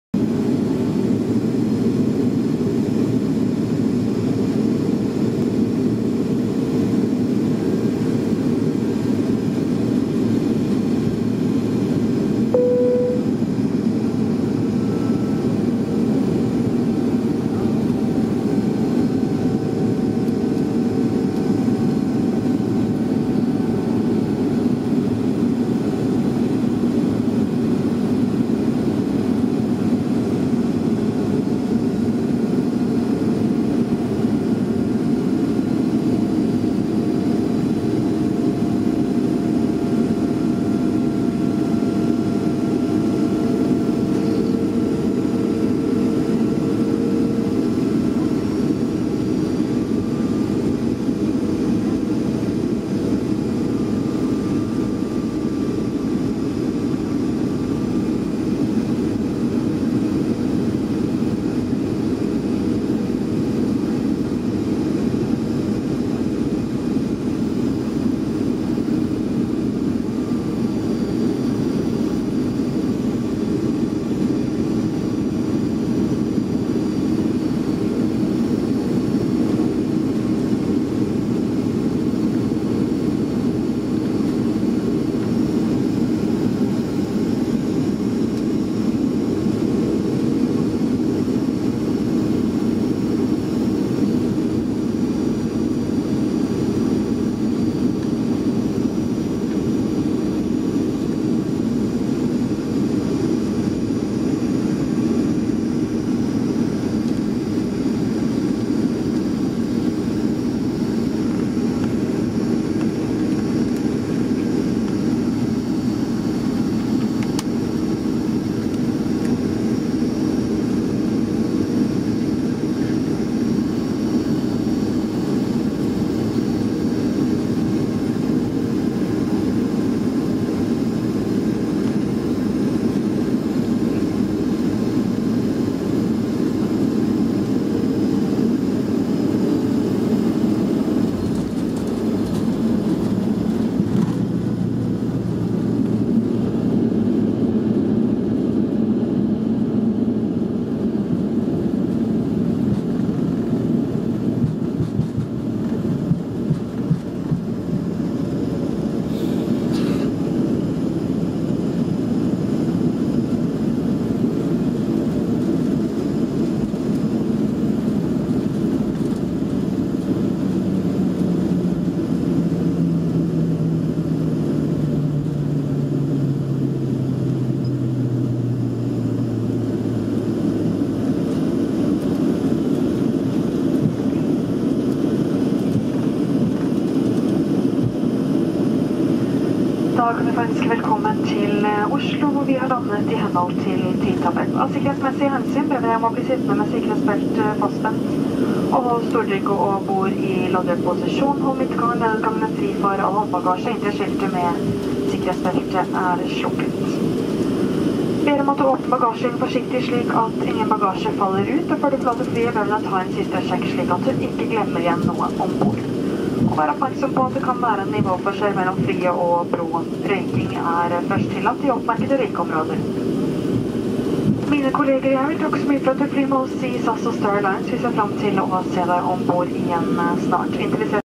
Final descent and landing in Oslo (OSL), Runway 19R
Flight SK 4756, 2017/10/01
Frankfurt (FRA) - Oslo (OSL), Runway 19R
Final descent and landing in Oslo (OSL)
Boeing 737-4784BC, Callsign LN-RRC